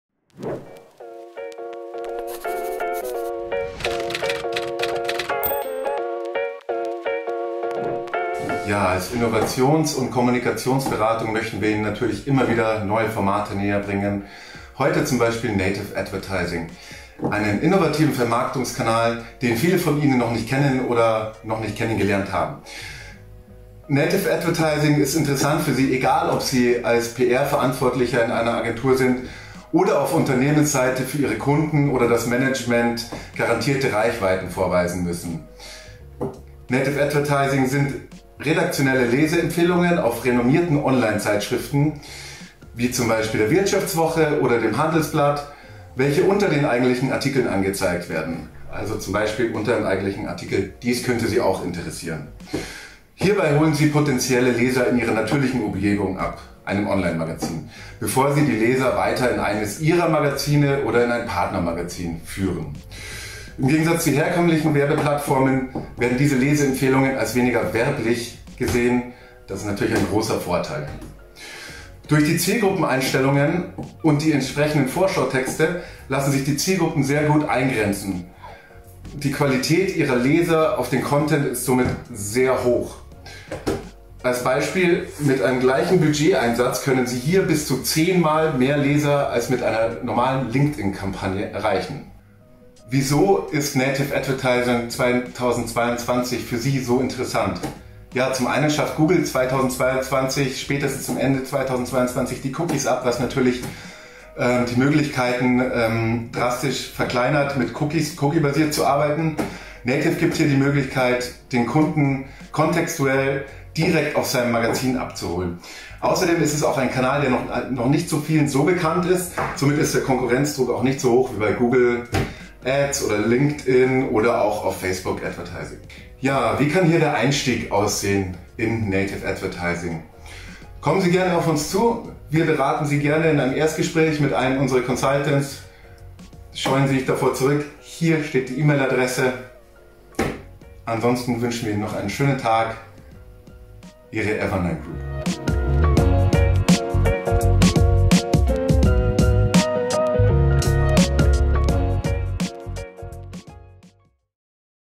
Evernine Talks. Native Advertising.
Folge #4 unseres Formats "Evernine Talks." Wir zeigen Ihnen, warum Marketing-Verantwortliche auf den noch viel zu selten eingesetzten Vermarktungskanal "Native Advertising" setzen sollten und geben dazu einen kurzen Überblick, was sich hinter diesem Instrument verbirgt, wo der Unterschied zu Google Ads, LinkedIn & Co. liegt und was Sie von einer Native-Advertising-Kampagne erwarten können.